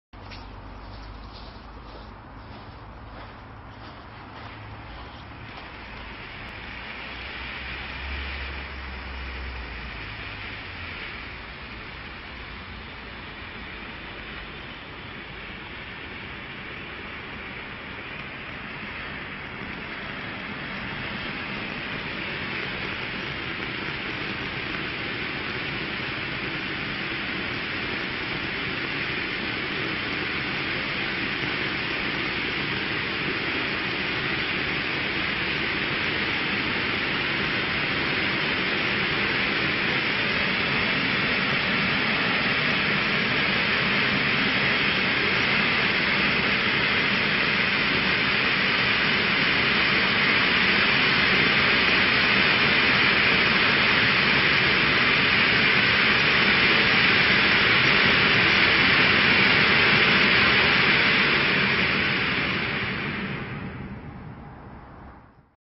Silber Vulkan(Zuckerstock) 60Sekunden
Schweizer Vulkan - Silber

Mit einer Effekthöhe von ca. 6 Metern, gehört der Silber Vulkan zu den beliebtesten seiner Klasse.

Bei einer Brenndauer von rund 60 Sekunden spuckt dieser Vulkan silberfarbene Effekte.

    20 cm
    Silber
    Brenndauer ca. 60 sec.

    Effekthöhe ca. 6 Meter